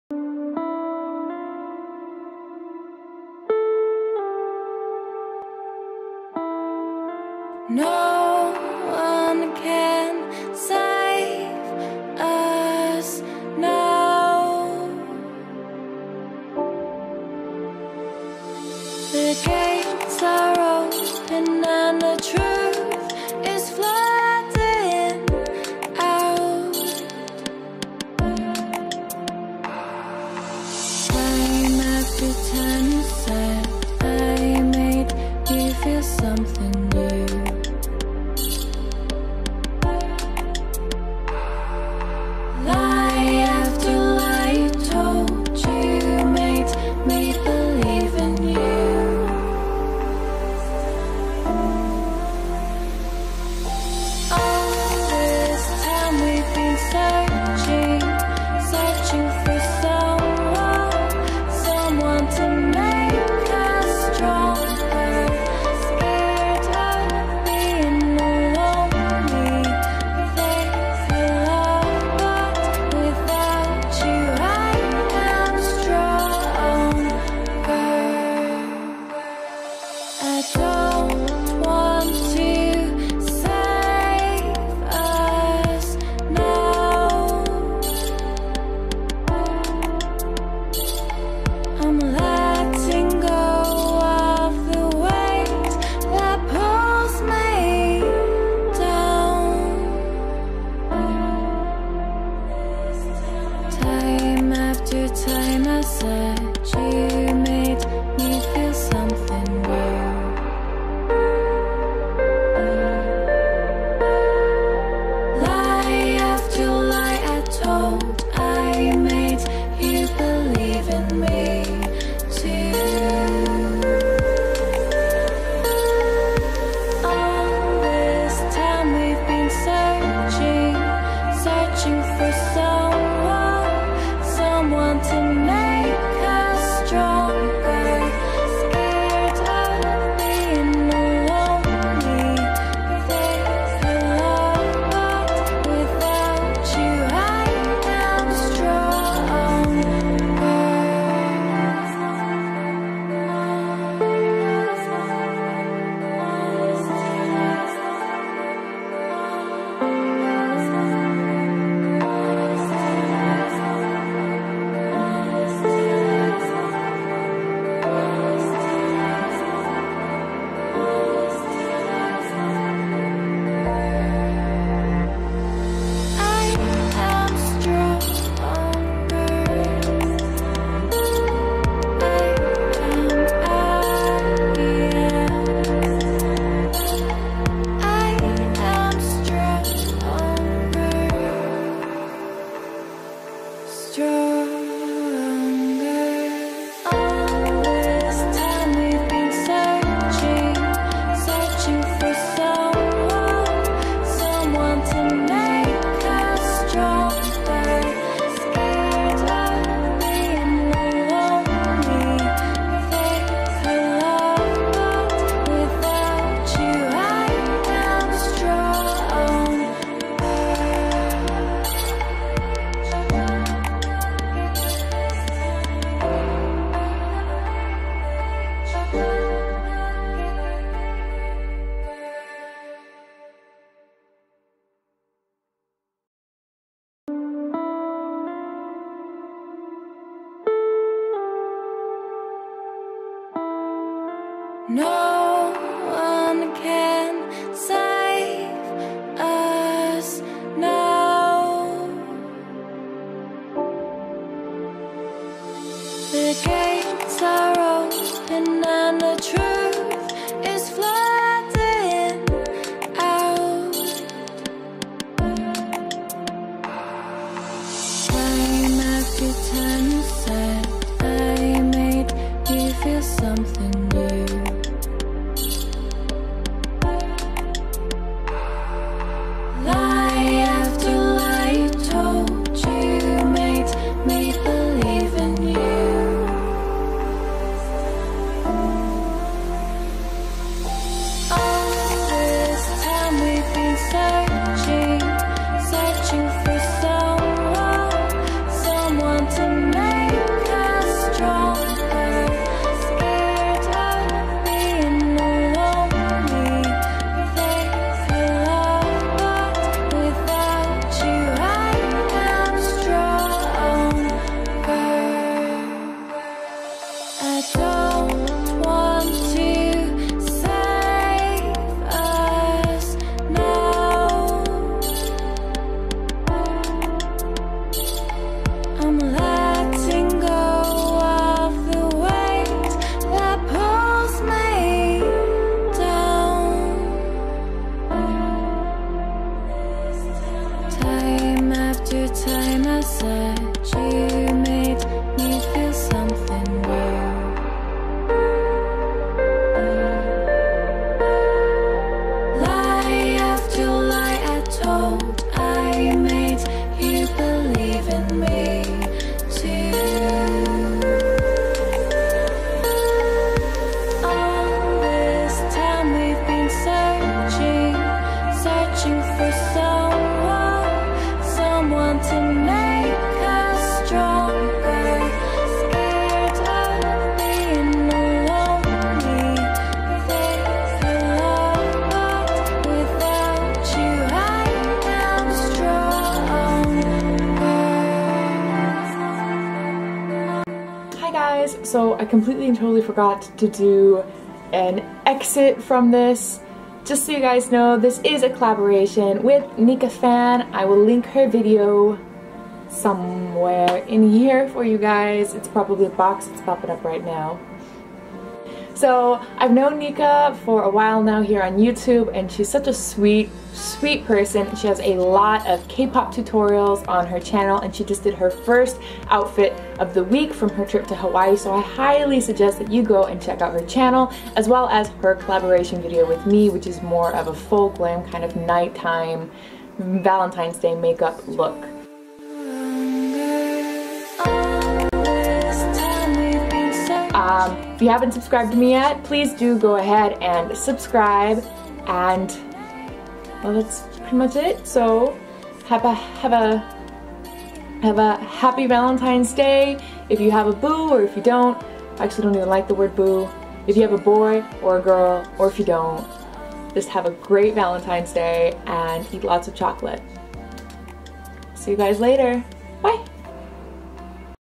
Valentines Day VS Night Date Makeup | Collab with NicaPhan!
Hey guys! So Nica  and I collaborated to make a day vs. night date makeup!  We opted to use the Naked 3 palette because of its romantic pink/blush tones and availability in the USA and South Korea!  Check out this video for an easy and sweet daytime date makeup look. Want something with a little more smoke and sultry? Head on over to Nica's channel and watch her video for a sexy evening date look!

Nica is a super sweet and funny gal from Cali who does a lot of Kpop tutorials on her channel as well, so make sure to check them out!

Products used:

| Eyes |

Maybelline Color tattoo
Just Beige

Urban Decay naked 3 Pallette
Strange
Nooner
Mugshot
Darkside

Etude house
Long Lash

Maybelline
Extreme Lash mascara
Black

| Brows |

Maybelline Color tattoo
Creamy beige

| Face |

Etude house
Face Blur Base

Etude House
Play 101 Duo Contour Stick

Innisfree
Mineral Foundation
W1

Etude House
Big Cover Cushion Concealer
Peach Pink

Luna
High Cover Concealer

Innisfree
Anti Sebum Powder

Innisfree
Shading Powder
#07 Sweet Vanilla

Innisfree
Marbelized Blush
#01 Pink Rose

| Lips |

Etude House
Auto liner
#02 Pink Beige

Essence
Lipstick
All about cupcake